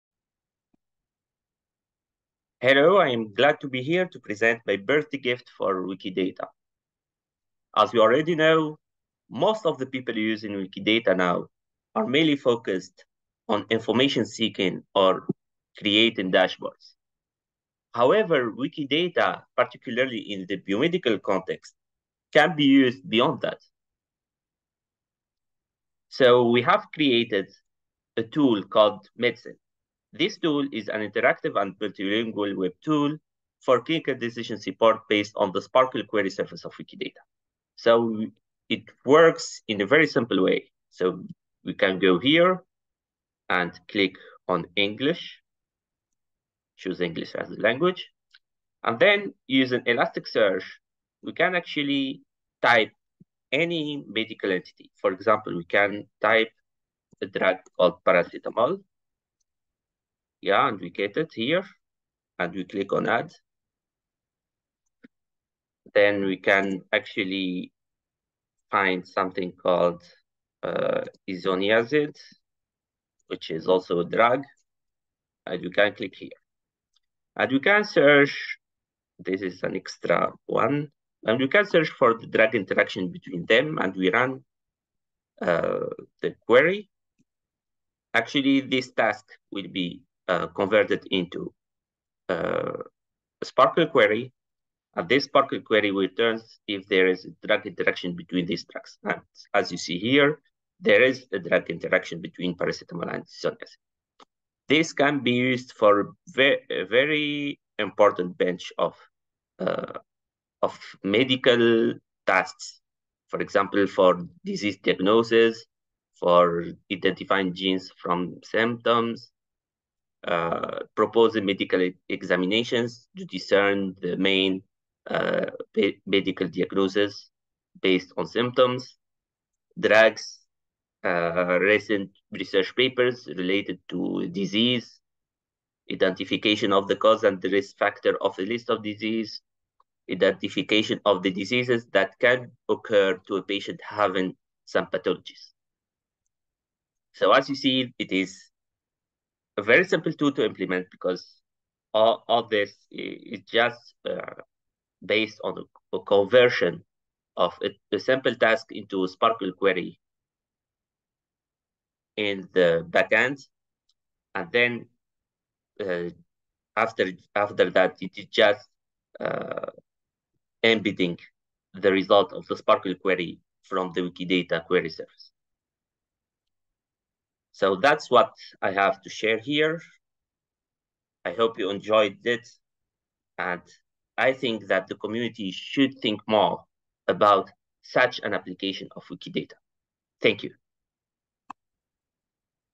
WikidataCon 2023 Birthday Presents - MedCYN: A Wikidata-driven clinical decision support web tool
MedCYN is an intuitive web tool for rule-based clinical decision support driven by SPARQL queries on Wikidata.